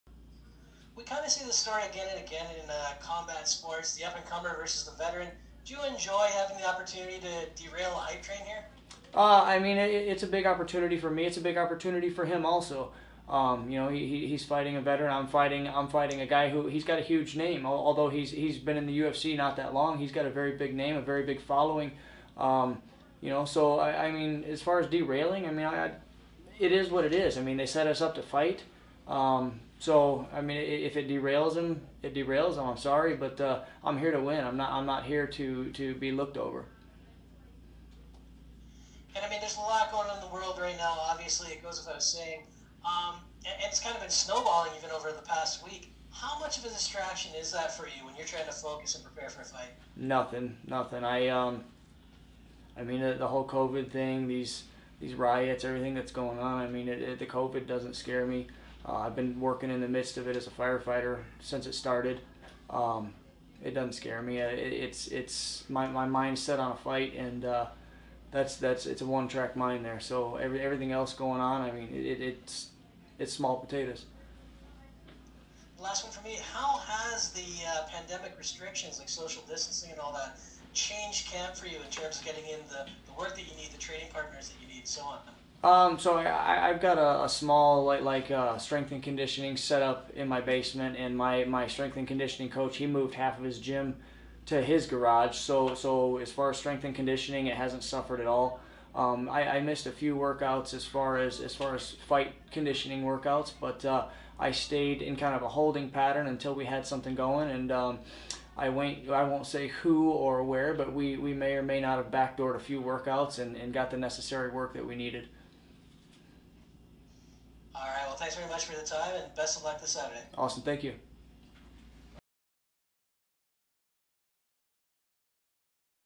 Eddie Wineland on O'Malley - "If I derail him, I derail him. I'm here to win" | UFC 250
Knowing that he is being underestimated in his match against Sean O'Malley, he expects that his experience will give him an edge while believing he is in better shape at 35 than he was at 25.